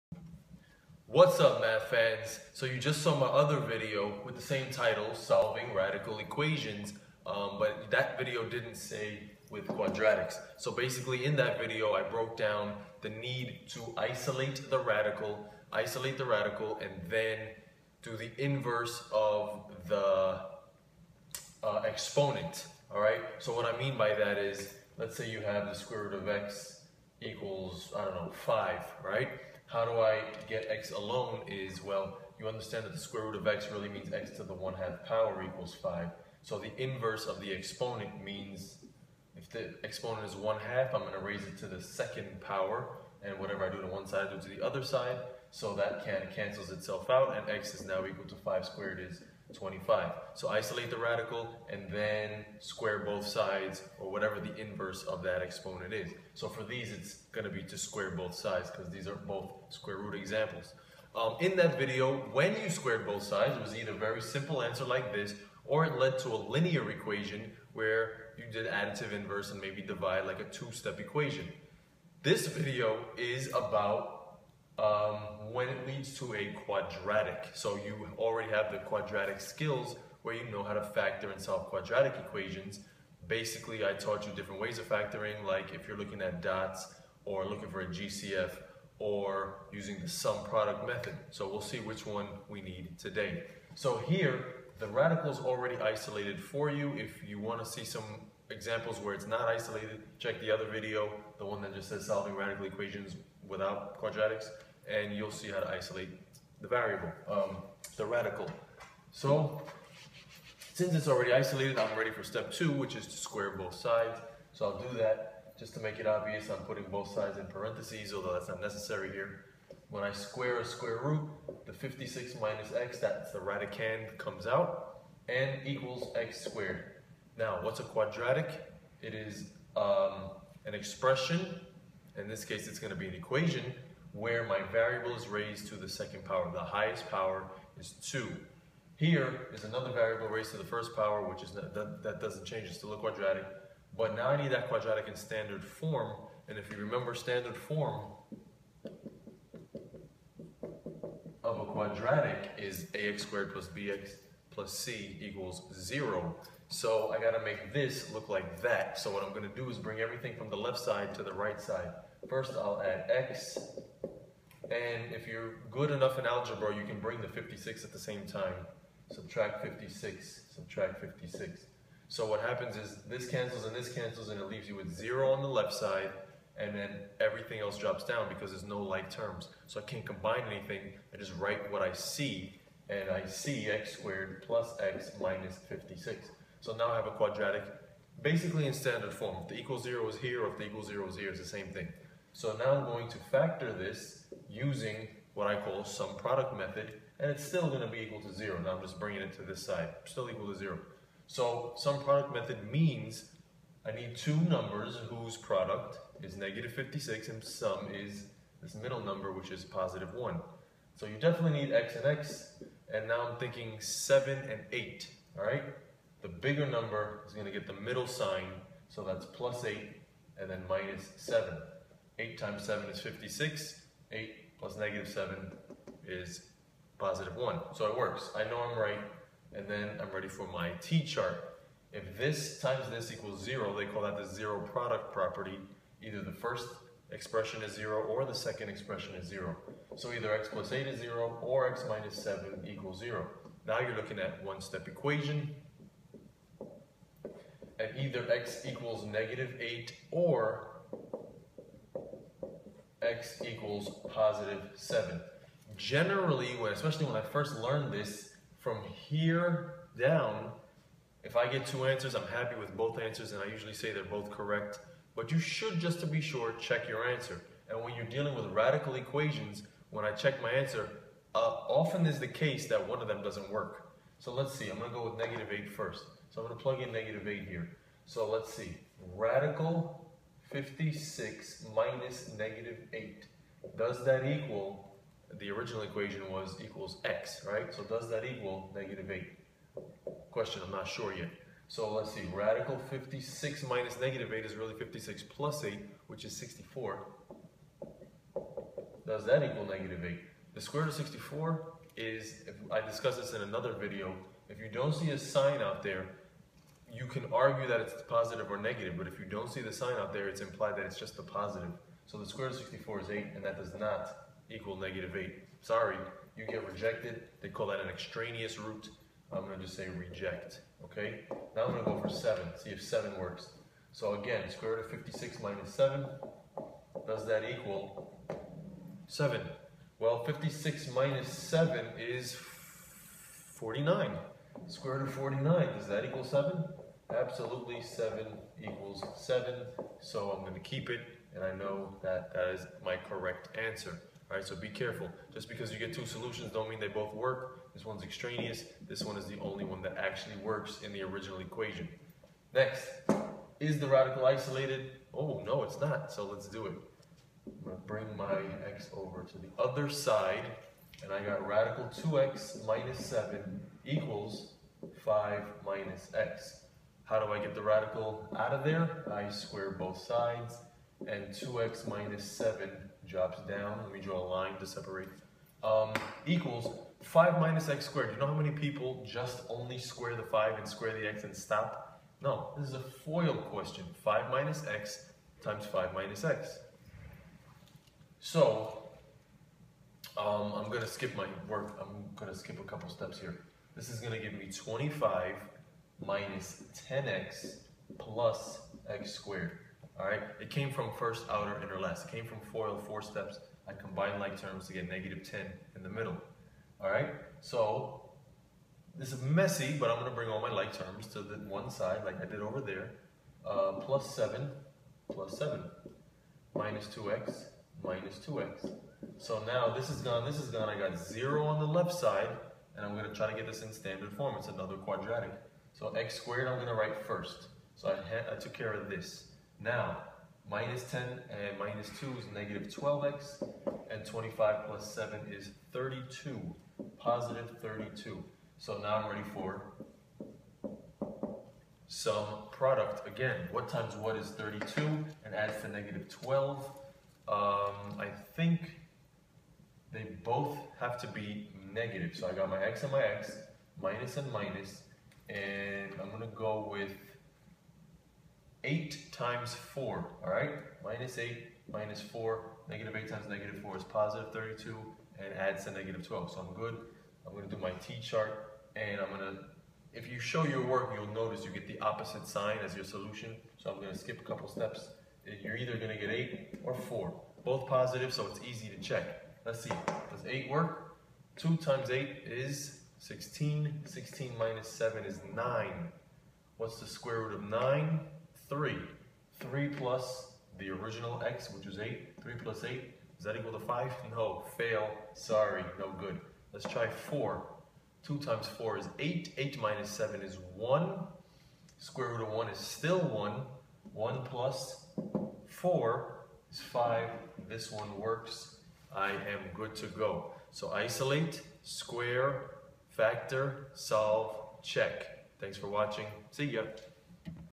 Solving Radical Equations (with quadratics)
Isolate the radical. Raise both sides of the equation to the necessary power (the reciprocal of the given power/radical), then if your variable is squared get the quadratic in standard form: ax^2 + bx + c =0. Then factor, use the t chart to solve and check to see if any solutions are extraneous.